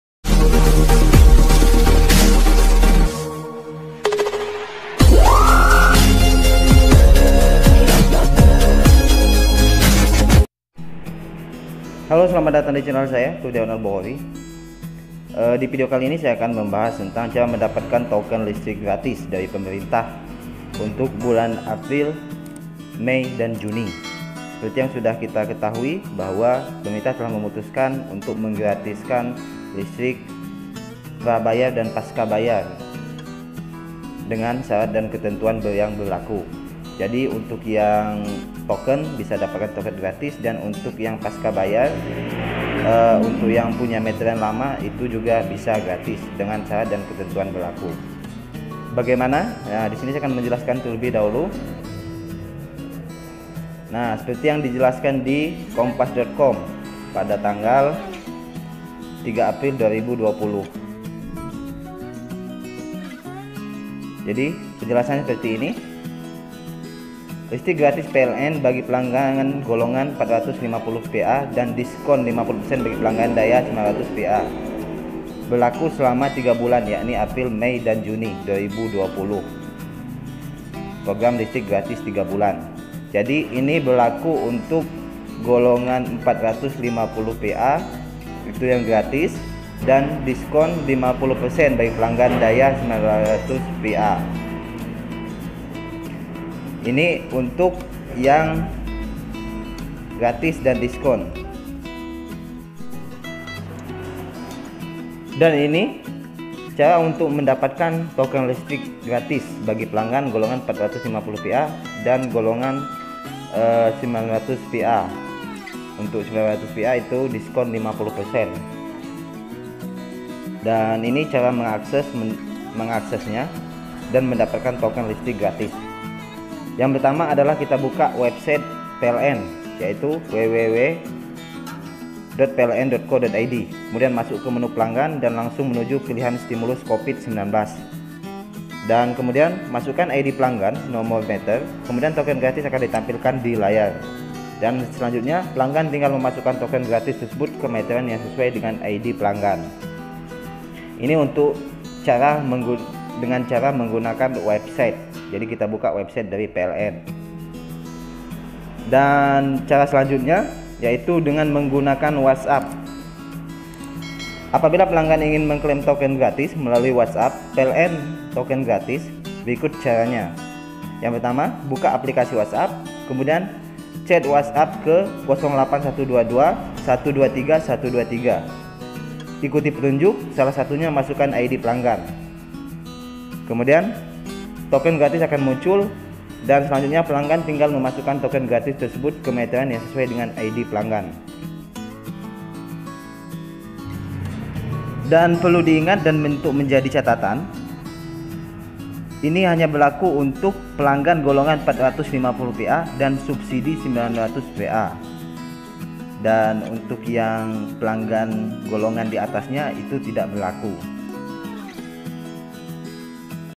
⭕Cara Dapatkan Token Listrik Gratis (Periode April, Mei, Juni 2020).. Bisa Klaim Di WhatsApp
Sumber : Kompas.com